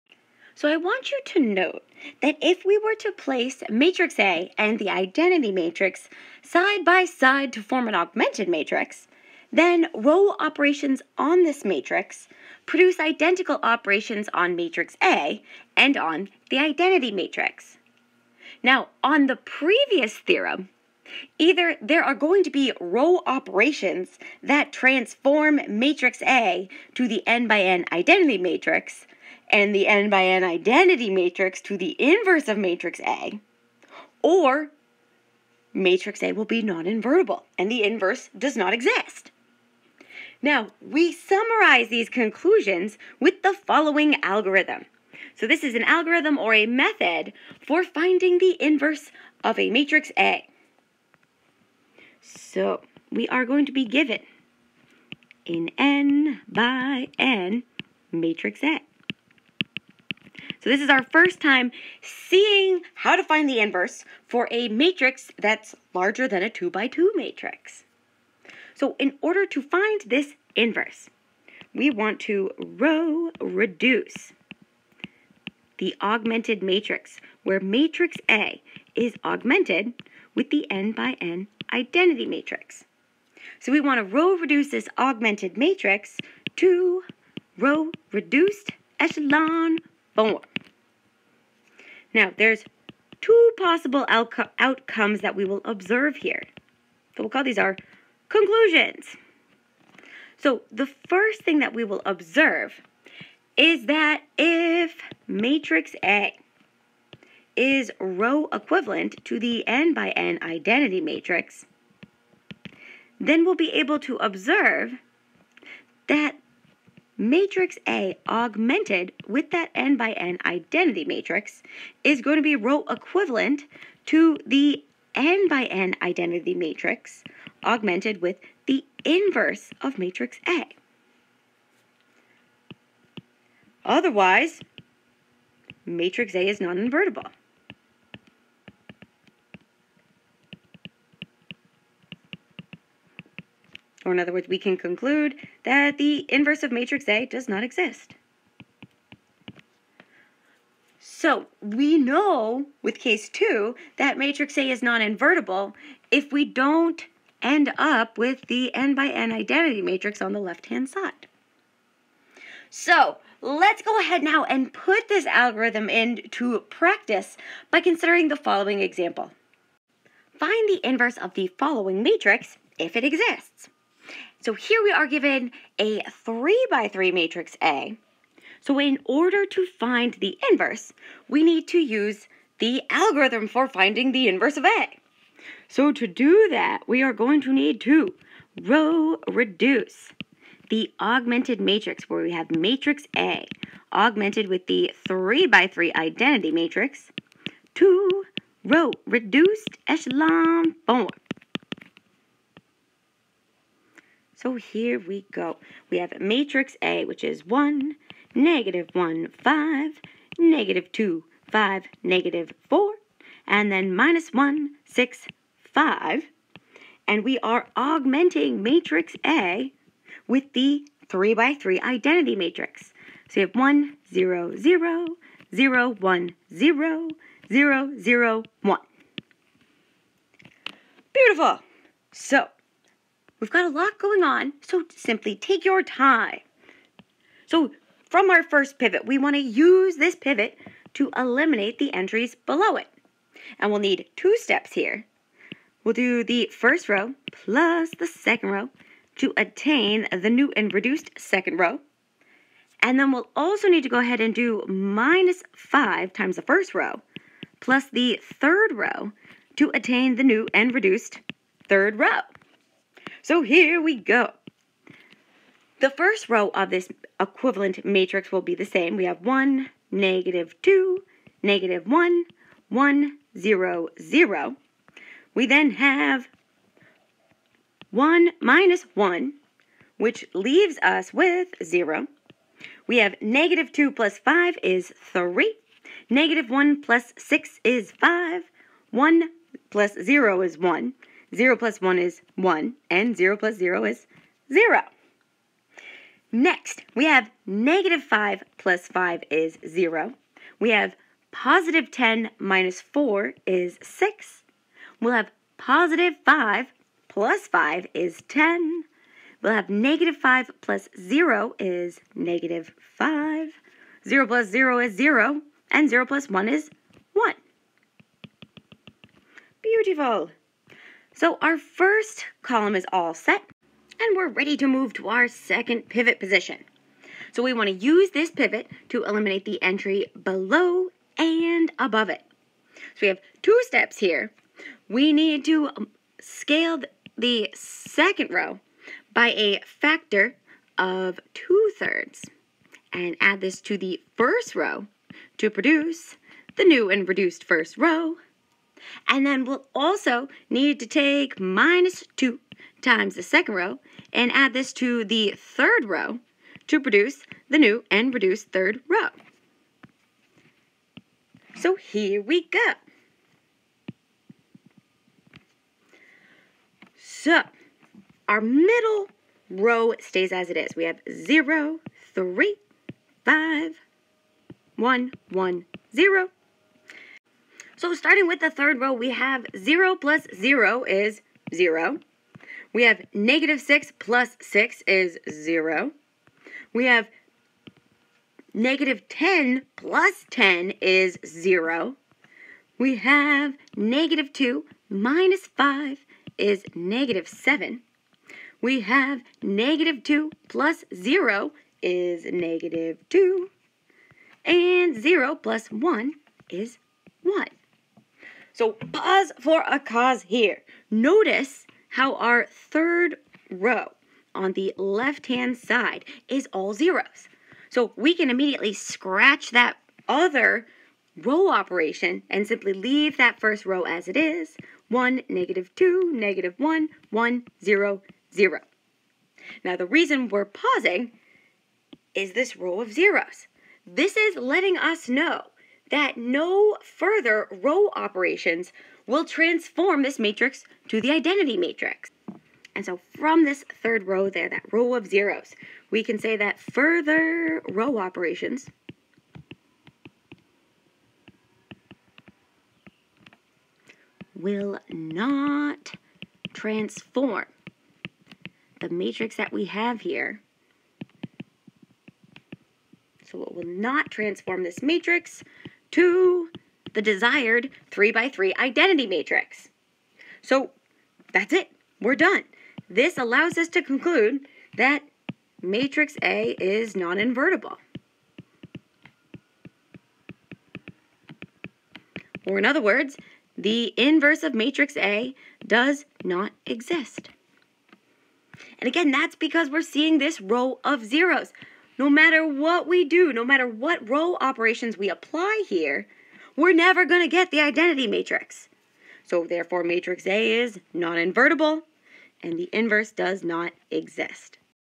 An Algorithm for Finding the Inverse (and Example 1)
Introduction & Illustration of an Algorithm for Finding the Inverse of a Matrix.

Example 1: Use the Algorithm to Find the Inverse of the Given Matrix if it Exists.